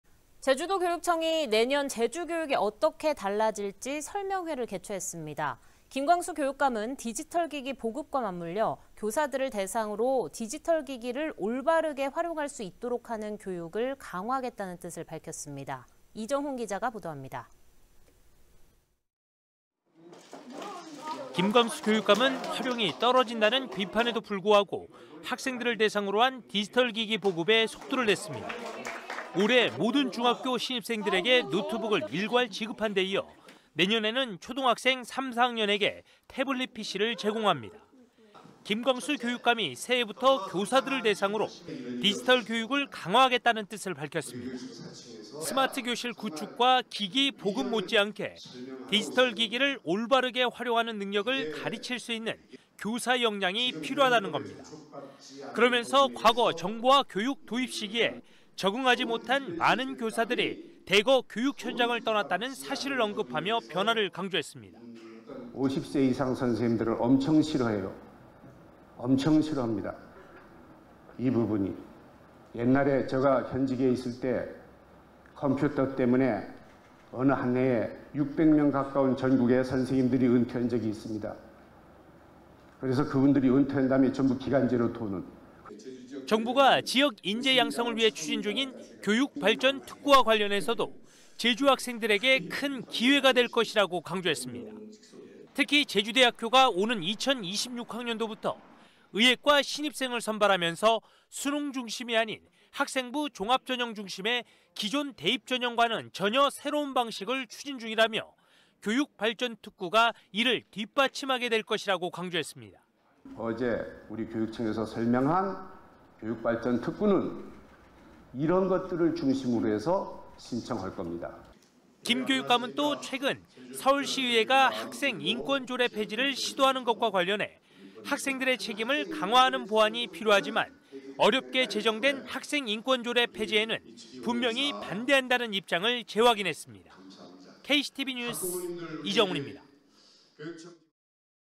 '교사들도 디지털 기기 교육'…리터러시 강화
내년에는 제주교육이 어떻게 달라질까요?

제주도교육청이 이에 대한 설명회를 개최했는데요.

김광수 교육감은 디지털 기기 보급과 맞물려 교사들을 대상으로 디지털 기기를 올바르게 활용하는 능력을 의미하는 '디지털 리터러시 교육'을 강화하겠다는 뜻을 분명히 했습니다.